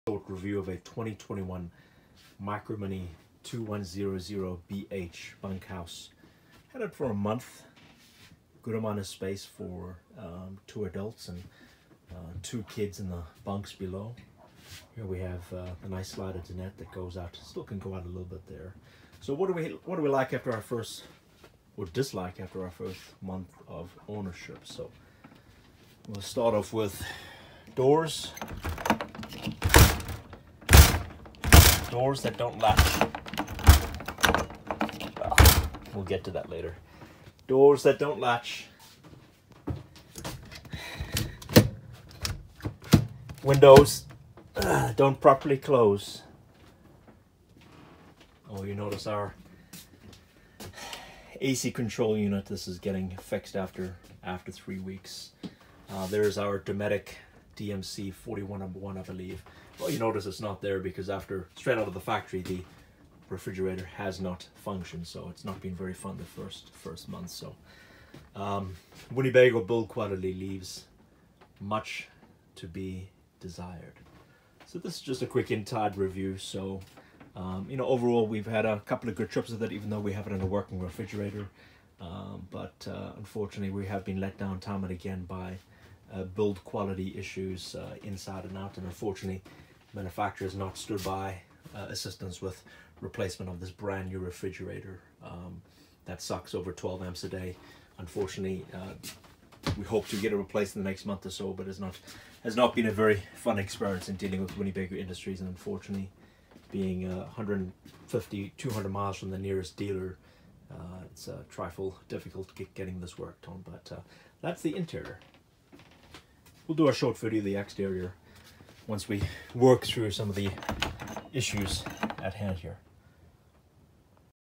Winnebago Micro Minnie 2100 BH
Quick interior review of MicroMinnie 2100 BH; DOMETIC DMC4101 problems from the factory.